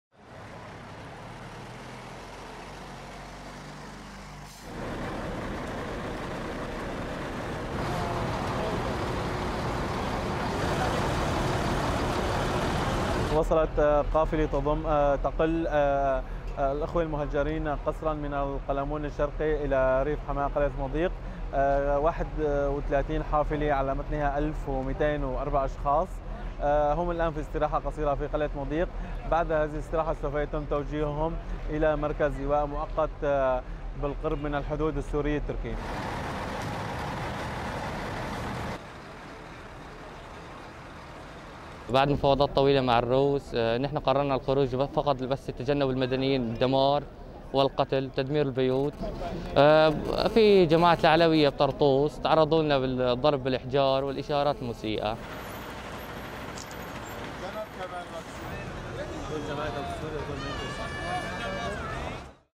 وصول الدفعة الثانية من مهجري منطقة القلمون الشرقي إلى قلعة المضيق بحماة
وصلت ليل الأحد – الاثنين 23 نيسان 2018، الدفعة الثانية من مهجري منطقة القلمون الشرقي شمال شرق العاصمة السورية دمشق، إلى معبر قلعة المضيق شمال غرب حماة، وسط البلاد. وتضم الدفعة 31 حافلة تقل نحو 1204 مهجرا من مدينتي الرحيبة وجيرود وبلدة الناصرية، حسب منسق الاستجابة في الشمال السوري عبيدة دندوش، والذي أضاف أنهم سينقلون المهجرين إلى مركز إيواء مؤقت بالقرب من الحدود السورية التركية". وبدوره أشار عنصر من حركة "أحرار الشام الإسلامية" أنهم تعرضوا لاعتداءات بالحجارة من قبل مؤيدي النظام أثناء مرورها في محافظة طرطوس وناحية بيت ياشوط باللاذقية غربي سوريا. وخرجت السبت، الدفعة الأولى من مهجري منطقة القلمون الشرقي لتصل لاحقا إلى ريف حلب الشمالي، حيث جاء ذلك تنفيذا لاتفاق توصل إليه الجيش الحر وروسيا، تضمن بنوداأبرزها نشر الشرطة الروسية في مداخل مدينتي الرحيبة وجيرود لضمان عدم دخول قوات النظام إليها، والحفاظ على أملاك المدنيين المهجرين وحقهم و"عدم مسها أو مصادرتها".